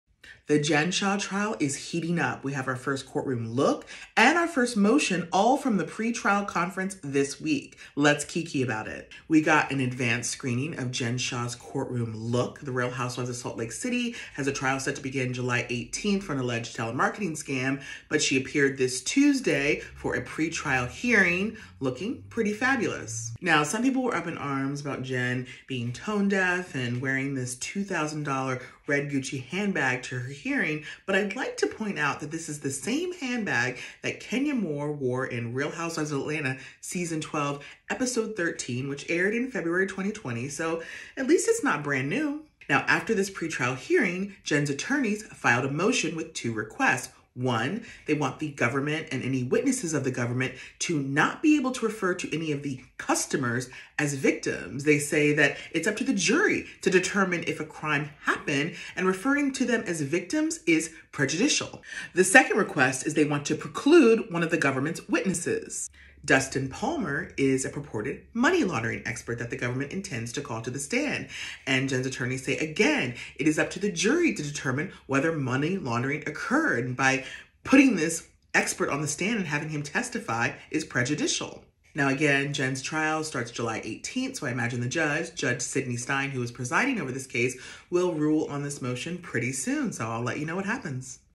Real Housewife Jen Shah's attorneys: "Don't call them victims!"
Salt Lake City Real Housewife, Jen Shah, appeared for a pre-trial hearing ahead of her July 18th trial start and her fashion choices caused a stir, but the motion that was filed after the hearing was much bigger news as her team wants the judge to not allow the government or the government witnesses to refer to customers as victims. They also want one of the government witnesses to not be allowed to testify.